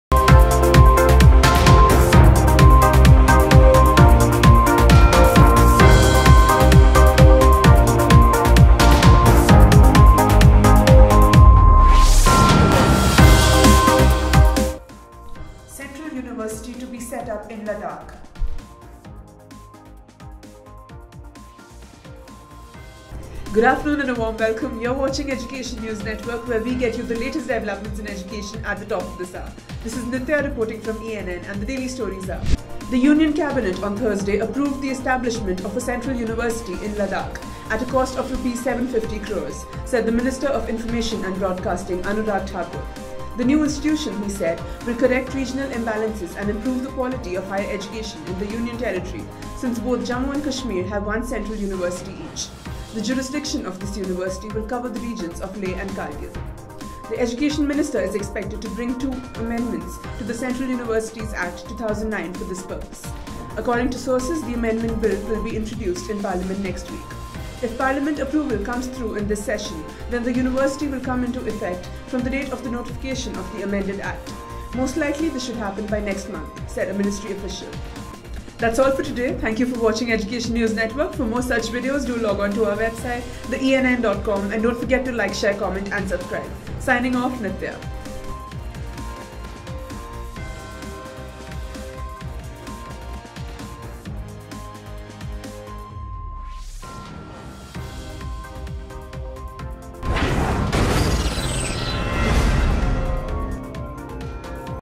Central University to be set up in Ladakh! | Education Today | Theenn.com
The Union Cabinet on Thursday approved the establishment of a central university in Ladakh at a cost of Rs 750 crore, said the Minister of Information and Broadcasting Anurag Thakur.
The new institution, he said, will correct regional imbalances and improve the quality of higher education in the Union Territory since both Jammu and Kashmir have one central university each. The jurisdiction of this university will cover the regions of Leh and Kargil.
The Education Ministry is expected to bring two amendments to the Central Universities Act, 2009 for this purpose. According to sources, the amendment Bill will be introduced in Parliament next week. “If Parliament approval comes through in this session, then the university will come into effect from the date of the notification of the amended Act. Most likely this should happen by next month,” said a ministry official.